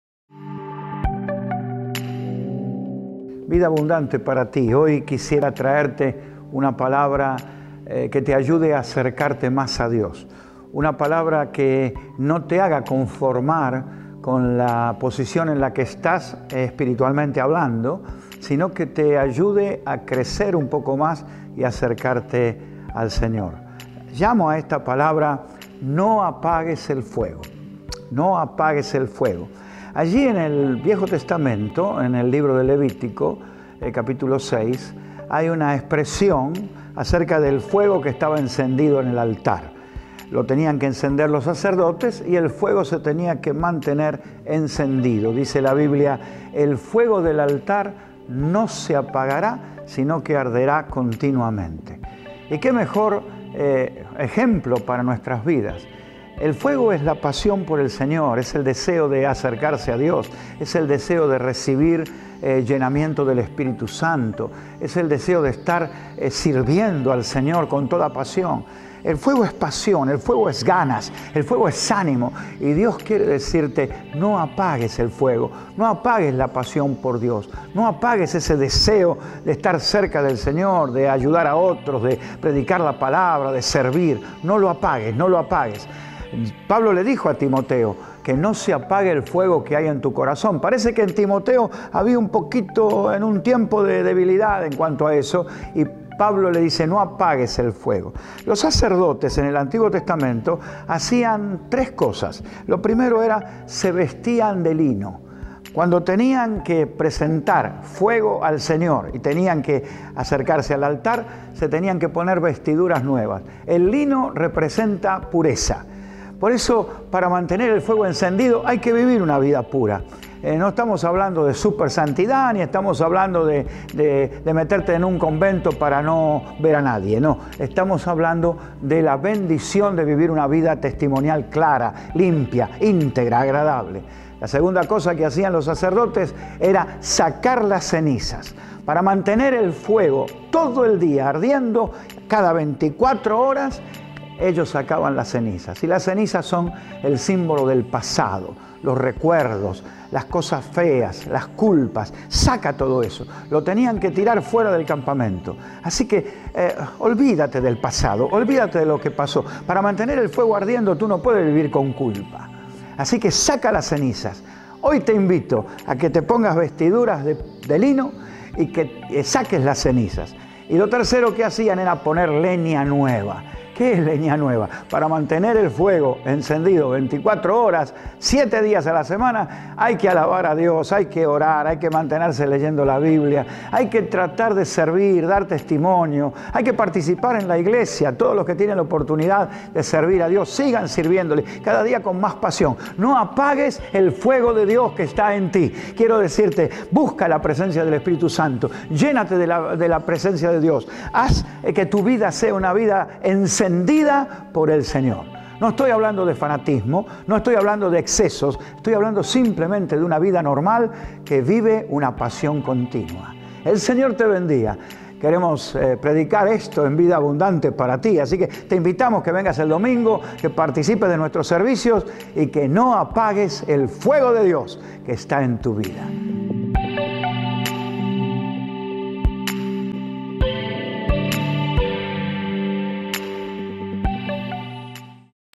Pastor Roberto Colloca "No Apagues El Fuego"
El Pastor Roberto Colloca te anima a mantener el fuego ardiendo en tu vida. vidamiami — with Roberto Colloca.